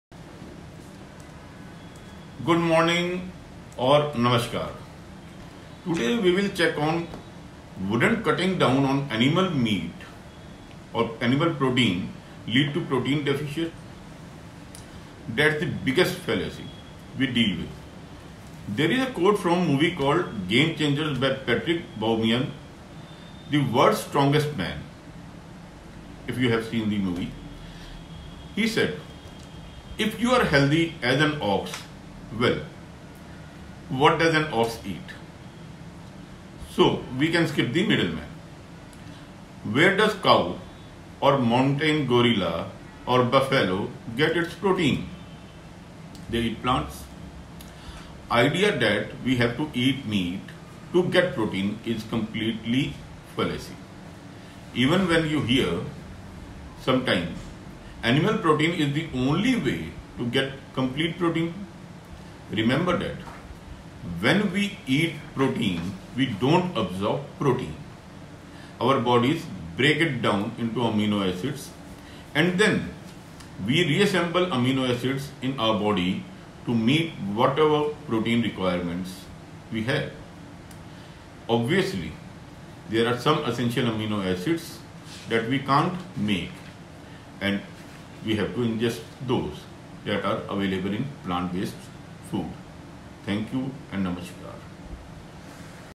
Wouldn’t cutting down on animal proteins lead to protein deficits? Know from Dr. Rajat Trehan
FOR MORE INFORMATION PLEASE CALL ON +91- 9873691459 OR WATCH DR. RAJAT TREHAN YOUTUBE CHANNEL.